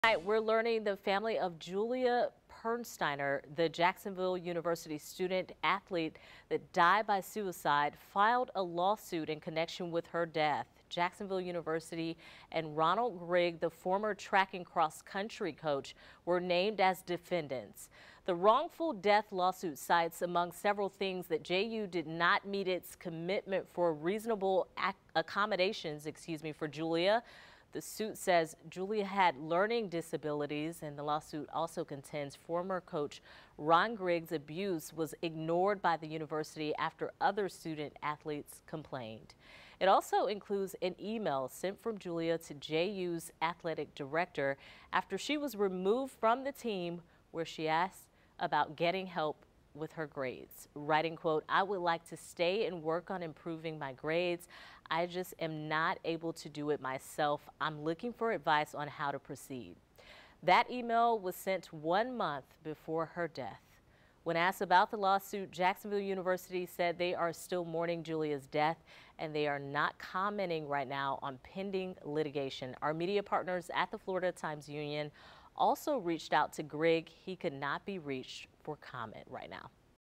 Lawsuit over former JU runner's suicide claims Title IX, ADA violations, complaints ignored
Julia Pernsteiner, 23, died in her dorm room in November 2021, less than a year after she transferred from the University of Pikeville in Kentucky.